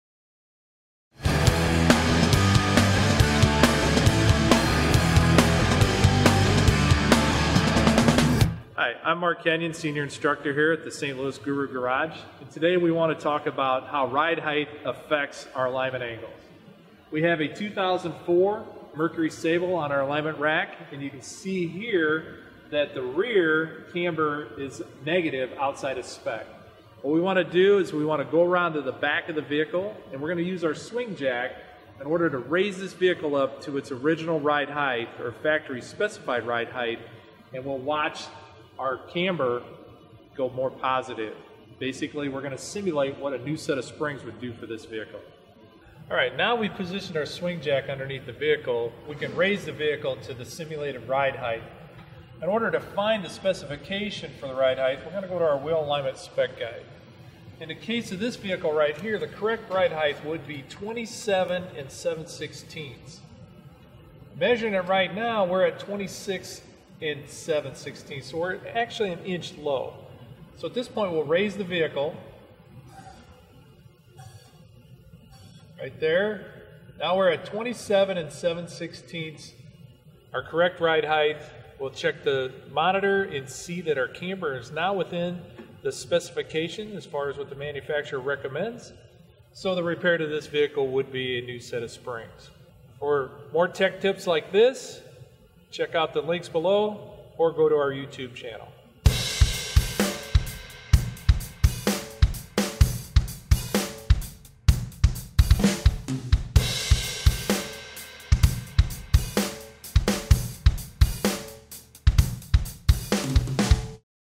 2004 Mercury Sable Ride Height Simulation
To learn more about diagnosing alignment problems and steering and suspension issues, visit Garage Gurus fmgaragegurus.com.

It is no secret that ride height has a direct impact on a vehicle's alignment. In this video, Master Technician Mark Kenyon of Garage Gurus demonstrates this with a ride height simulation on a 2004 Mercury Sable. With the vehicle on the alignment rack, Mark shows on the computer that as he brings the ride height back into the manufacturer specification, the vehicle returns to proper alignment. Count on Garage Gurus to help you get your vehicle repair done right.